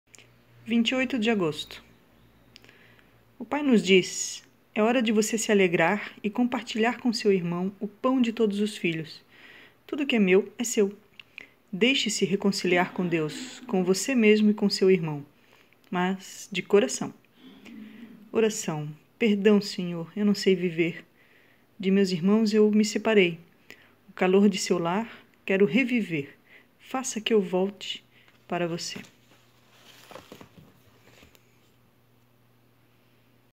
Os cinco minutos com meu amigo Francisco - Agosto - dia 28
Pequena oração diária do Papa Francisco

Livro Os cinco minutos com meu amigo Francisco - Agosto - dia 28

28/08
28 de Agosto